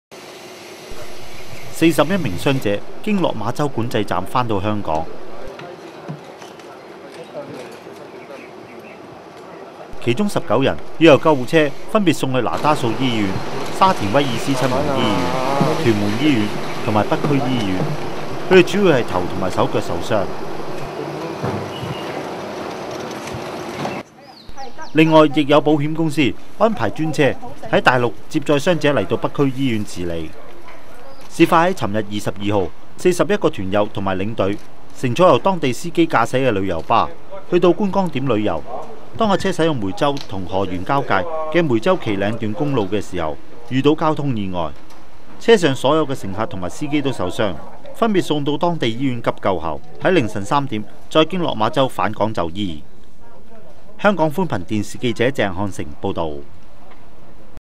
本港旅行團廣東遇車禍41傷
本港一個旅行團在廣東梅州遇到交通意外，四十一個人受傷，凌晨經落馬洲返港就醫。